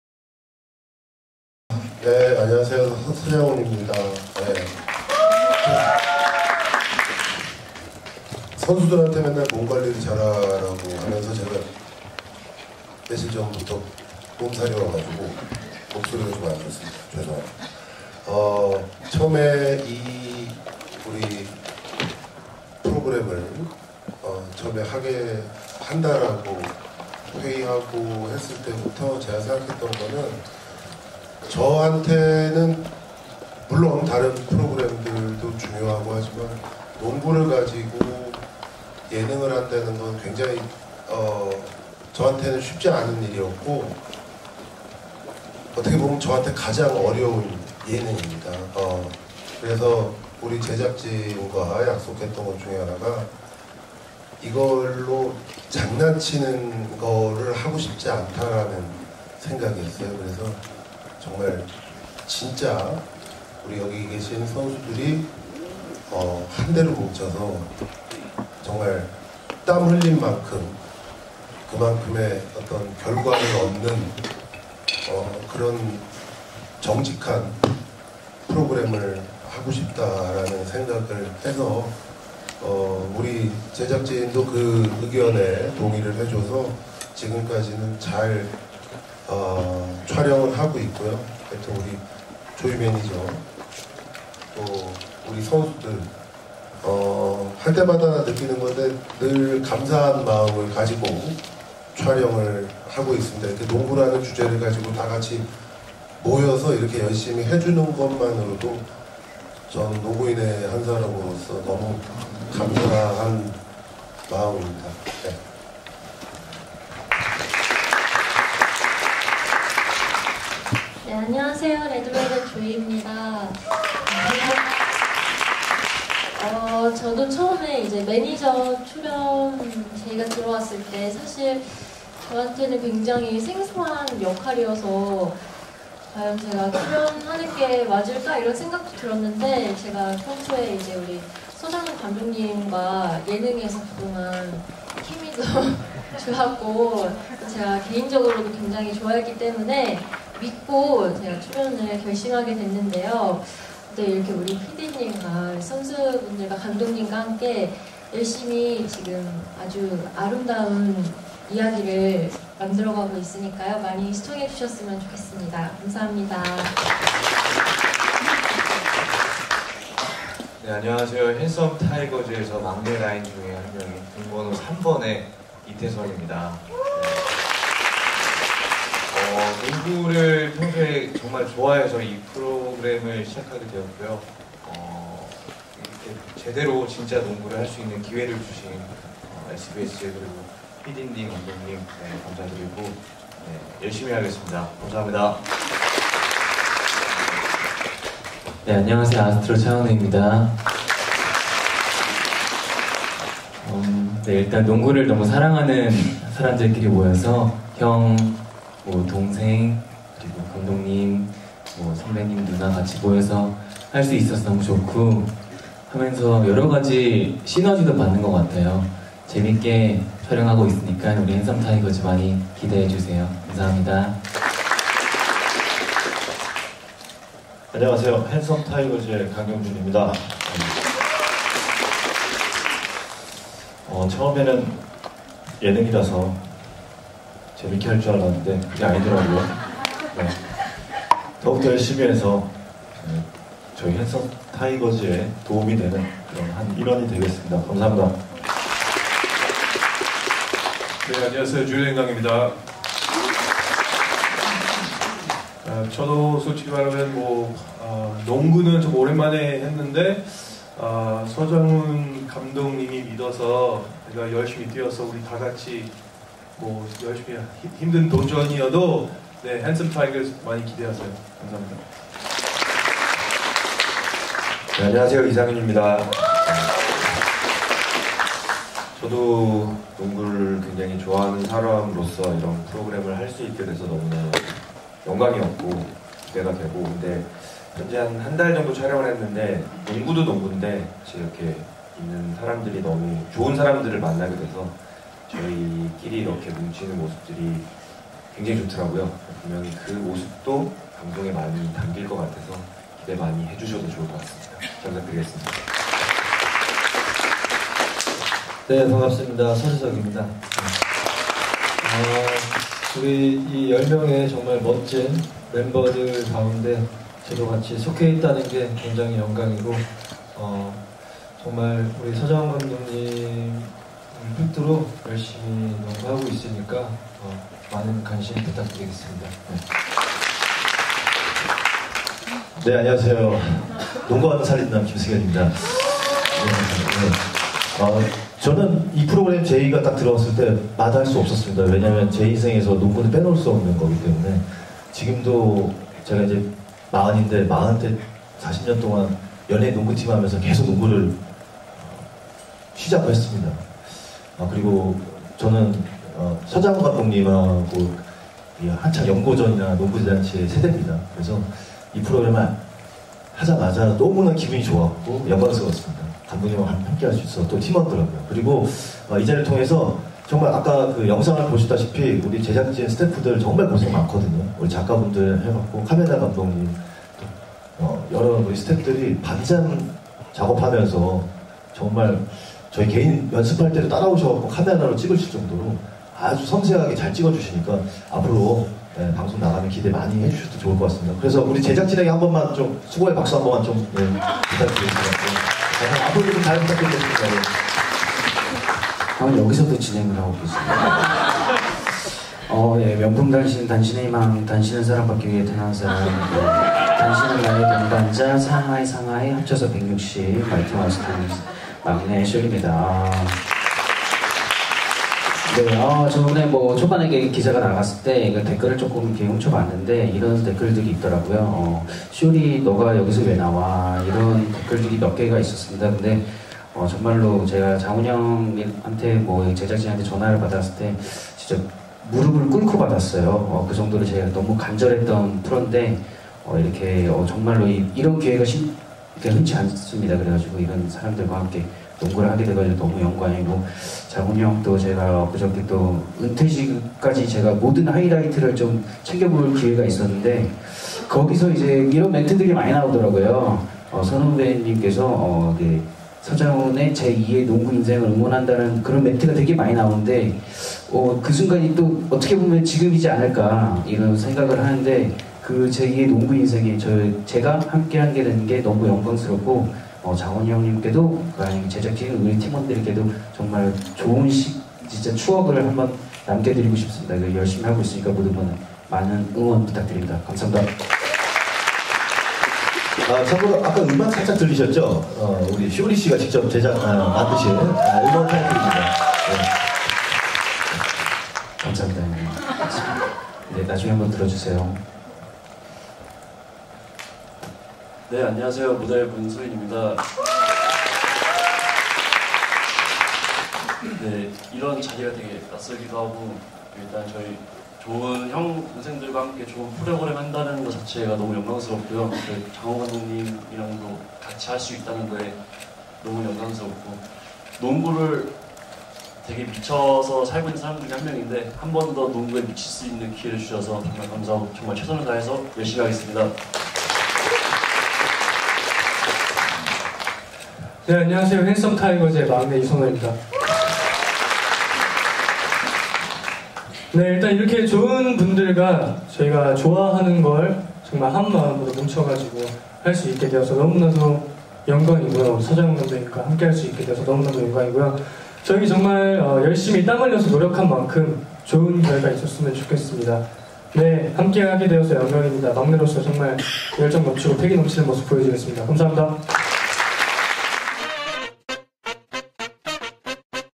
200109 핸섬 타이거즈 감독과 선수 자기소개 (SBS '진짜 농구, 핸섬타이거즈' 제작발표회)
9일 오후 서울시 양천구 목동 SBS에서 2020 SBS 새 예능 ‘진짜 농구, 핸섬타이거즈’ 제작발표회가 열려 감독 서장훈, 매니저 레드벨벳 조이, 선수 차은우, 유선호, 이상윤, 서지석, 김승현, 줄리엔 강, 쇼리, 강경준, 문수인, 이태선이 참석해 자기 소개를 하고 있다.

SBS 새 예능 ‘진짜 농구, 핸섬타이거즈’는 ‘한국 농구계의 전설’ 서장훈이 농구 감독을 맡고, 선수로는 이상윤, 서지석, 김승현, 줄리엔 강, 쇼리, 강경준, 문수인, 이태선, 차은우, 유선호가 레드벨벳 조이가 매니저로 활동하는 '진짜 농구, 핸섬타이거즈'는 10일 금요일 밤 11시 10분에 첫 방송 된다.